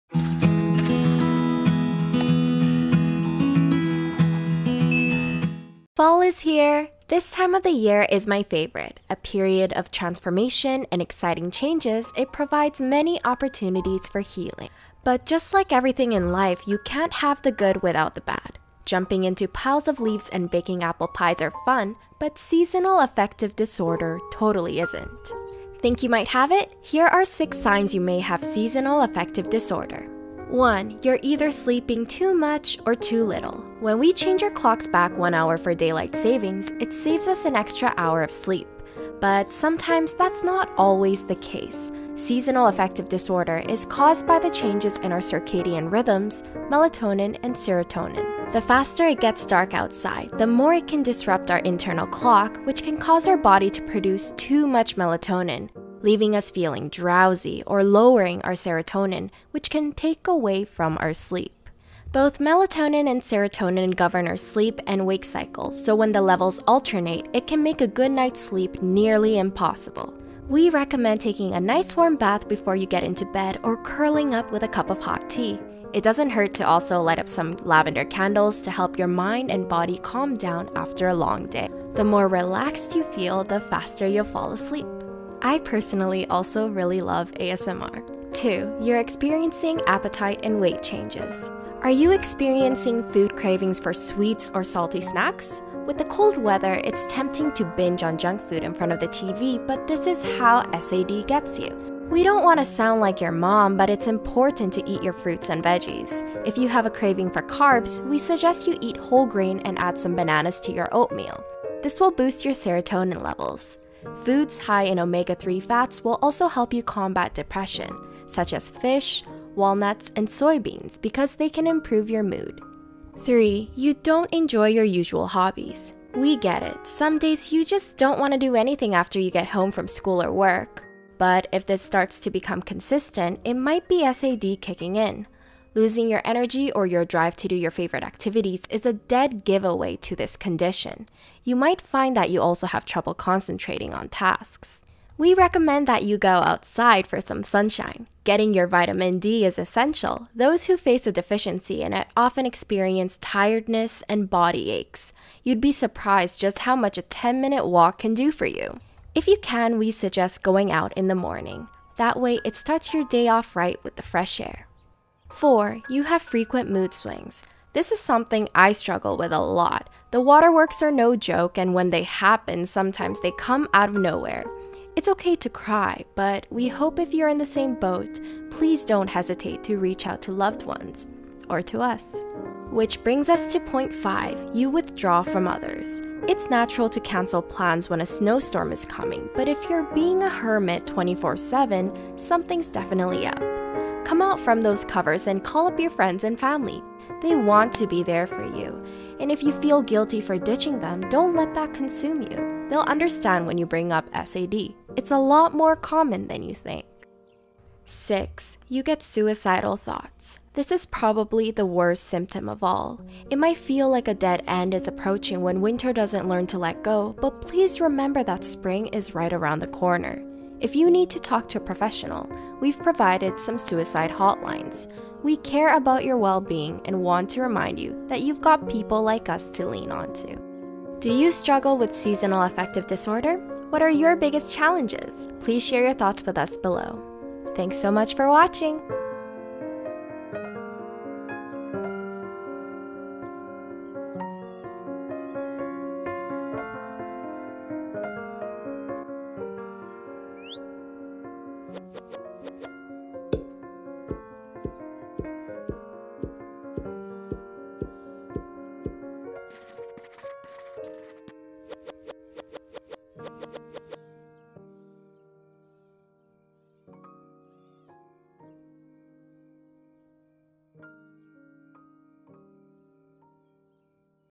6 Signs You Have Seasonal Affective Disorder (SAD)
Seasonal Affective Disorder, or SAD, is something many people suffer from. Learn about the signs and symptoms of Seasonal Affective Disorder - do you share any of these 6 signs or seasonal depression? Do you sleep too much or too little? Maybe you experience appetite and weight changes, too. Another sign is if you don't enjoy your usual hobbies. Perhaps you have frequent mood swings, or even withdraw from others. Do you struggle with seasonal affective disorder? What are your greatest challenges? Please share your thoughts with us below!

References:

Seasonal Affective Disorder (SAD). (2018). HelpGuide.org. Retrieved October 9, 2018.